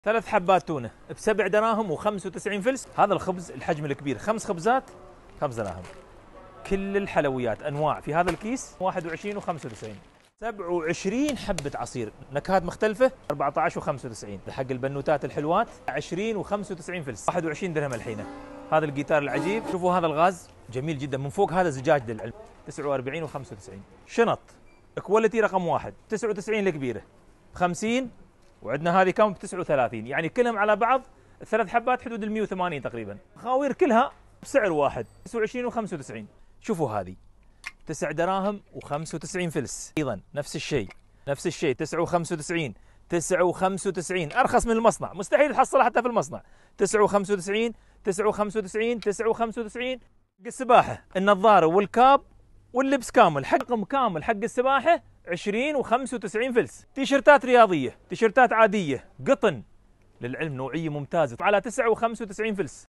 مركز القبائل للتخفيضات في عجمان واقوى العروض الصيفية على جميع السلع والادوات المنزليةلغاية ٢/٨/٢٠٢٣
مركز القبائل للتخفيضات في منطقة الروضة في عجمان واقوى العروض الصيفية على جميع السلع والمنتجات الغذائية والادوات المنزلية ولغاية ٢/٨/٢٠٢٣ والحق ما تلحق ✅🫶🏽🌹